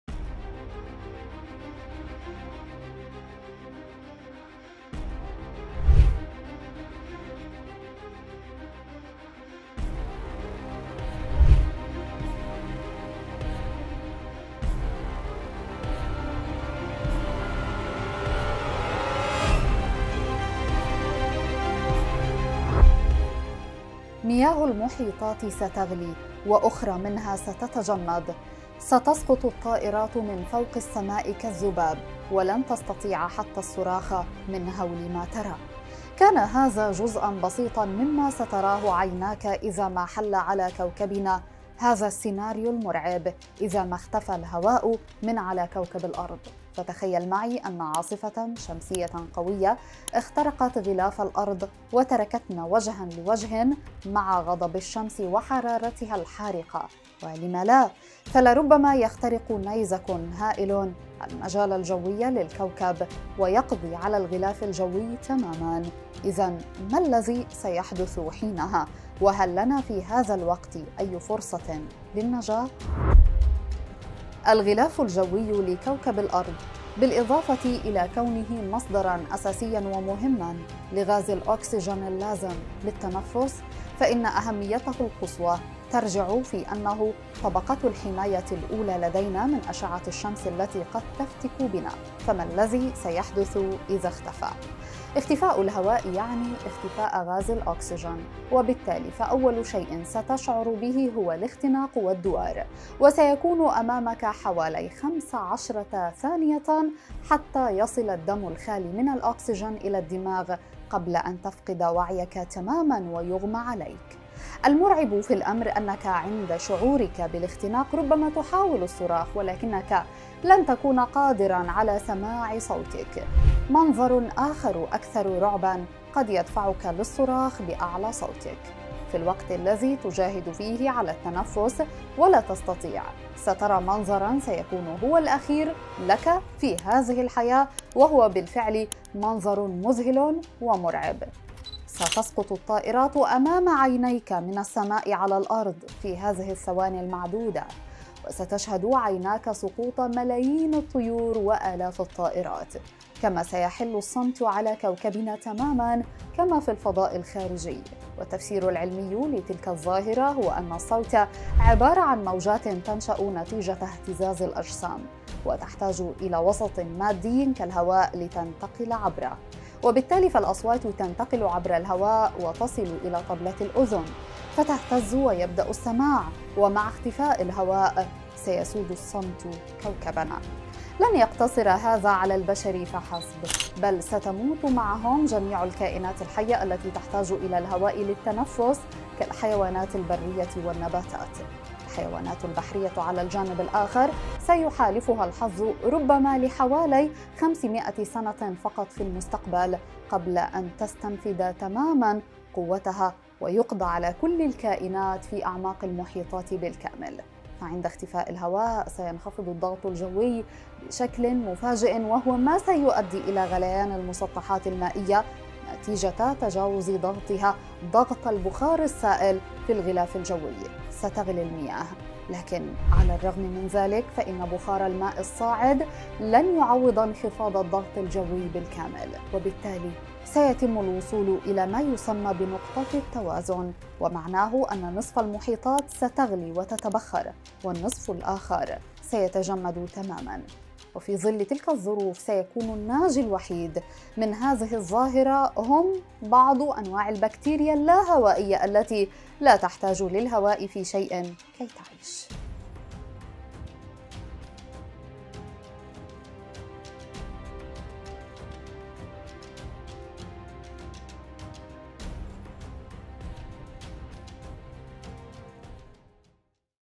ستتبخر مياه البحار والمحيطات وتتساقط الطيور من السماء ولن نستطيع الحراك.. ما الذي سيحدث ؟
مياه المحيطات ستغلي وأخرى منها ستتجمد، ستساقط الطائرات من فوق السماء كالذباب، ولن تستطيع حتى الصراخ من هول ما ترى.
كان هذا جزءا بسيطا مما ستراه عيناك إذا ما حل على كوكبنا هذا السيناريو المرعب، إذا ما اختفى الهواء من على كوكب الأرض.
فتخيل معي أن عاصفة شمسية قوية اخترقت غلاف الأرض وتركتنا وجها لوجهة مع غضب الشمس وحرارتها الحارقة، ولما لا فلربما يخترق نيزك هائل المجال الجوي للكوكب ويقضى على الغلاف الجوي تماما.